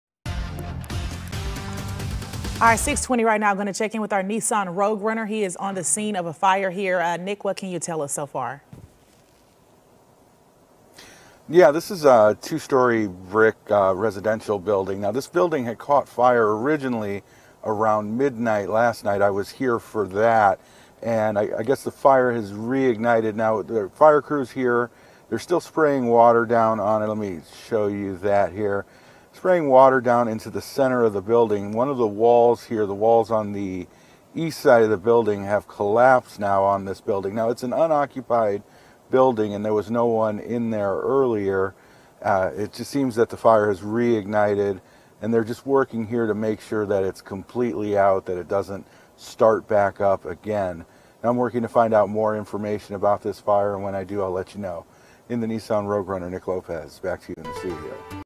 Crews responding to building fire in St. Louis City
Crews are responding to a building fire early Thursday morning.